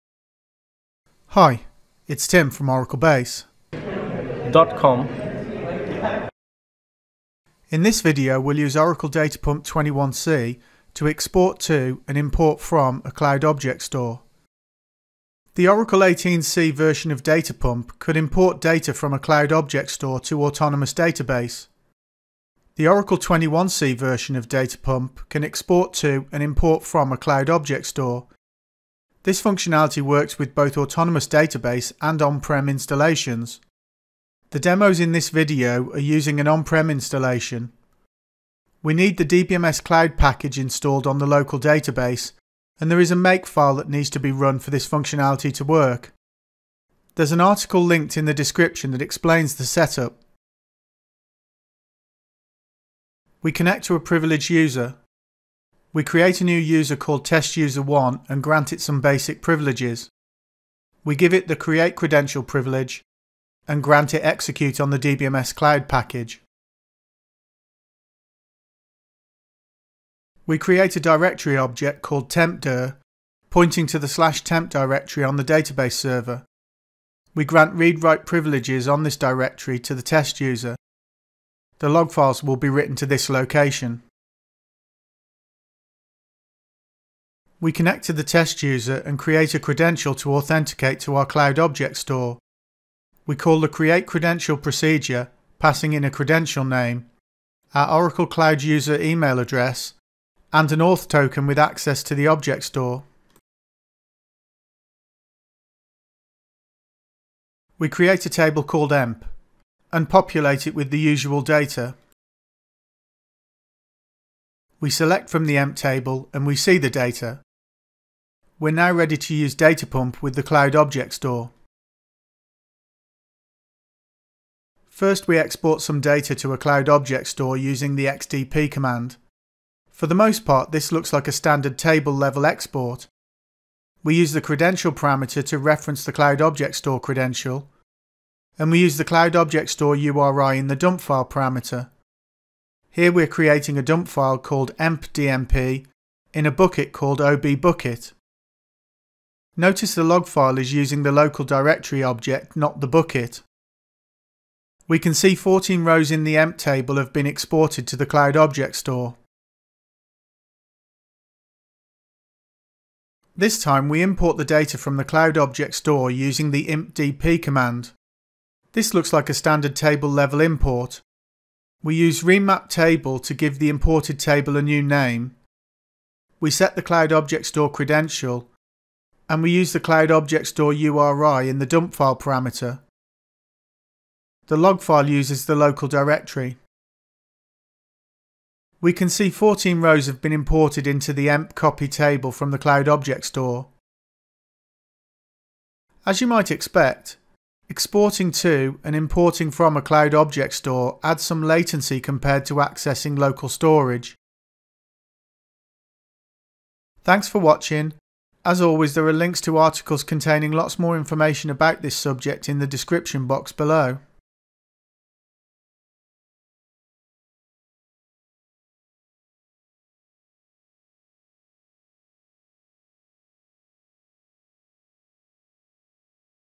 Oracle Data Pump 21c and Cloud Object Stores
In this video we'll use Oracle Data Pump 21c to export to, and import from, a cloud object store.

Data Pump Export (expdp) to and Import (impdp) From Cloud Object Stores in Oracle Database 21c

Oracle Cloud Infrastructure (OCI) : Create an Object Storage Bucket